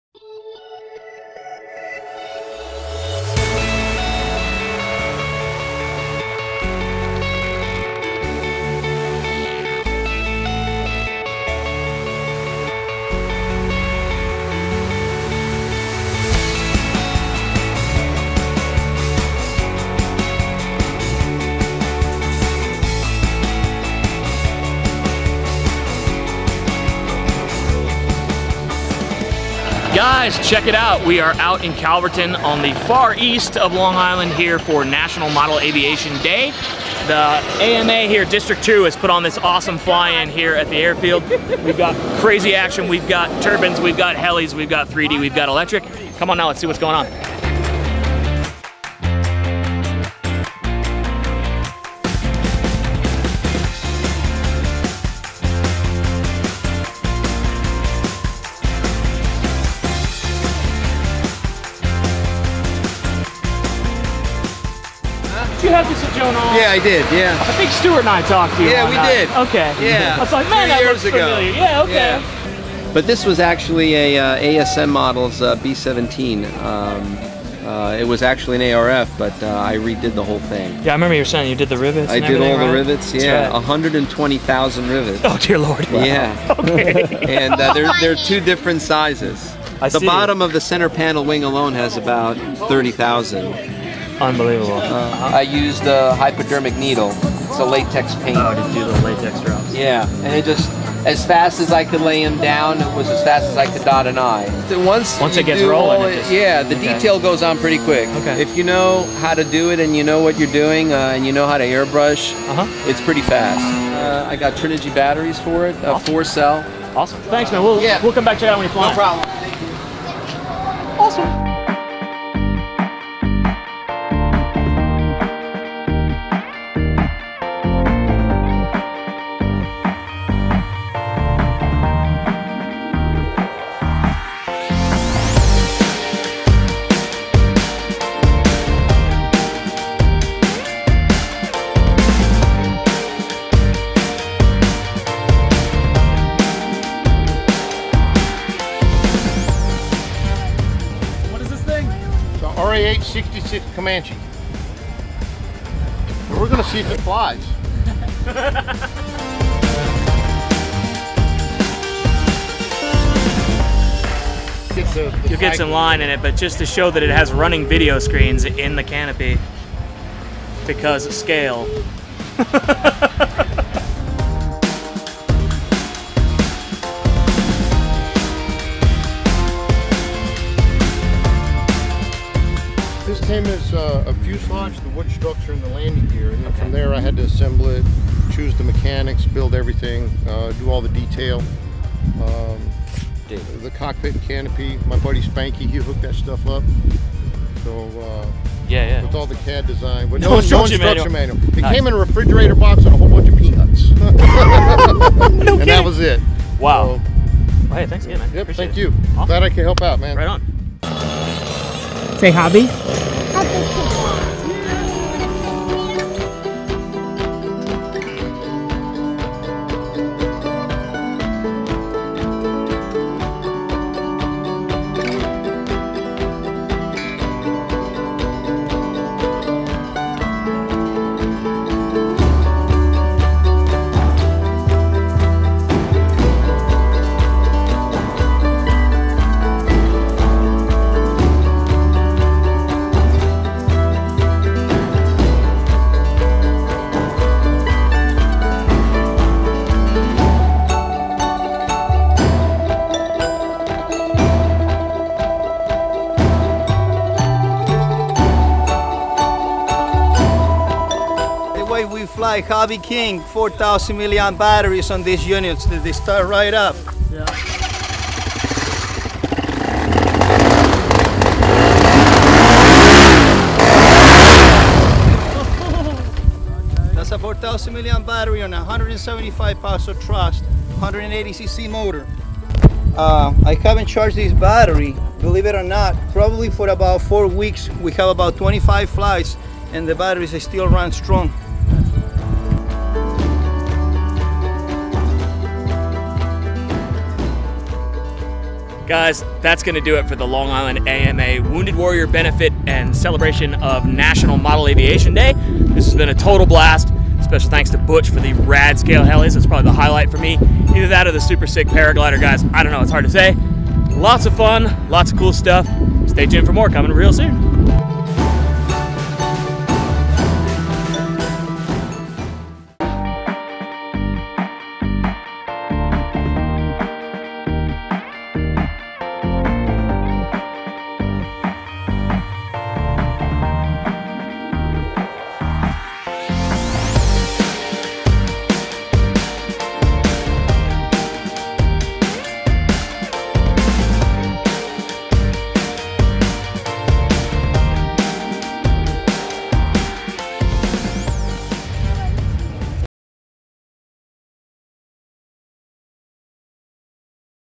HobbyKing Live - Long Island AMA Wounded Warrior Benefit
The HobbyKing US team had a fantastic time at the first annual Long Island AMA Wounded Warrior Benefit. We were proud sponsors and look forward to this event growing in future years.